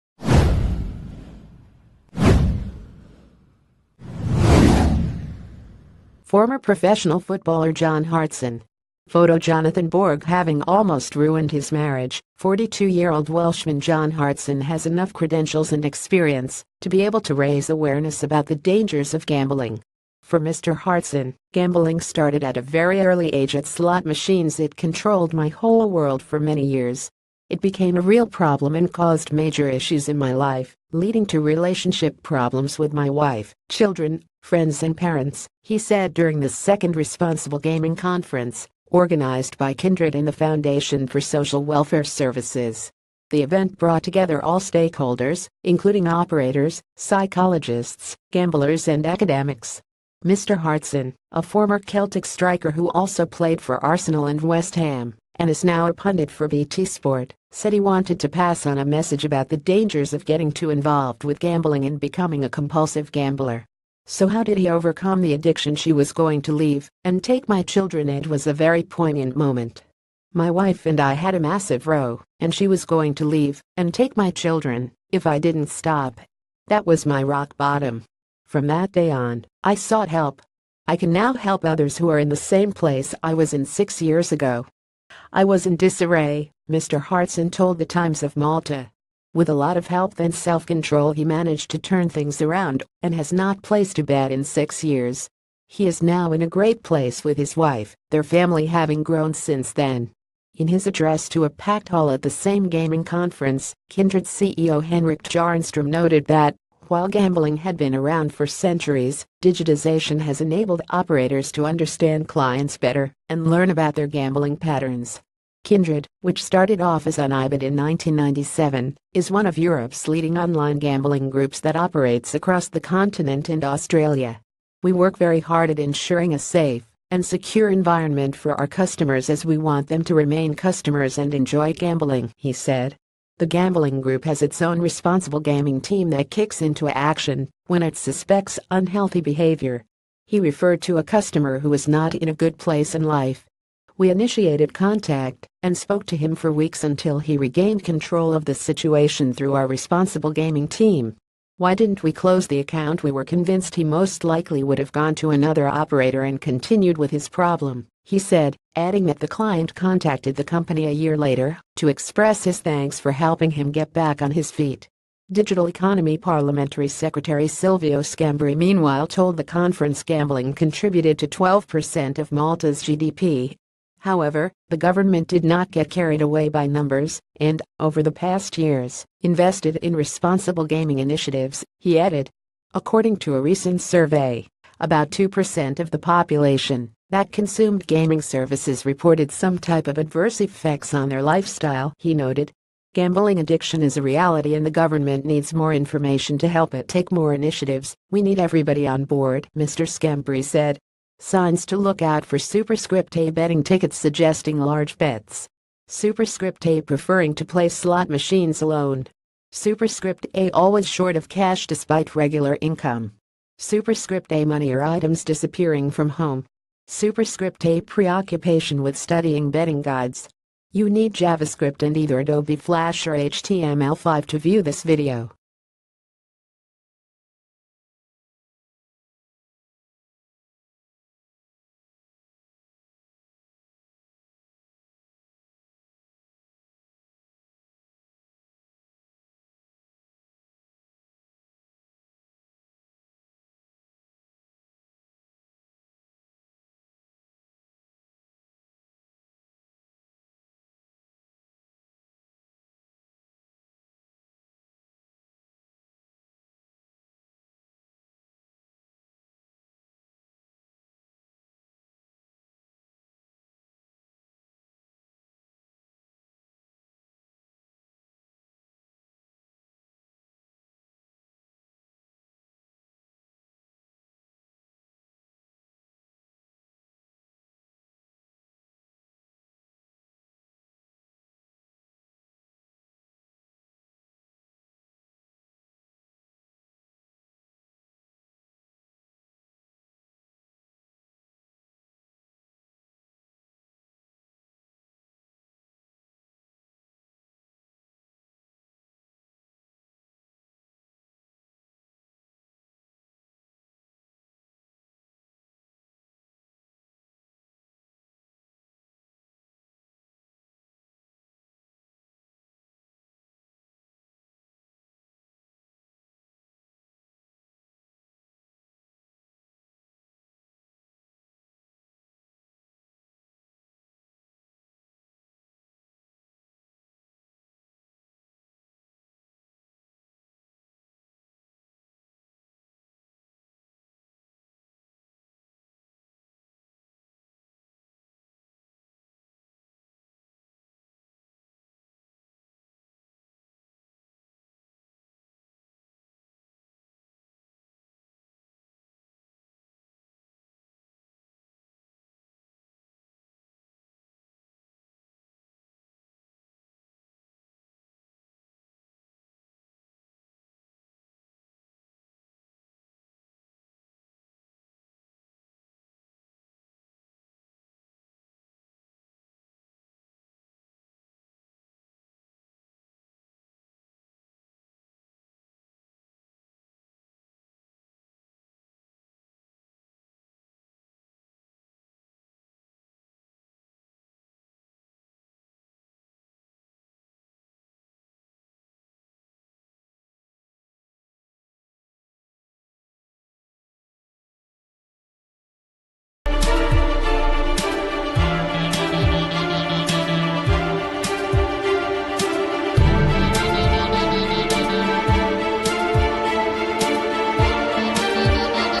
Breaking News | ‘gambling controlled my whole world for many years’
‘gambling controlled my whole world for many years’

Former professional footballer John Hartson. Photo Jonathan Borg Having almost ruined his marriage, ...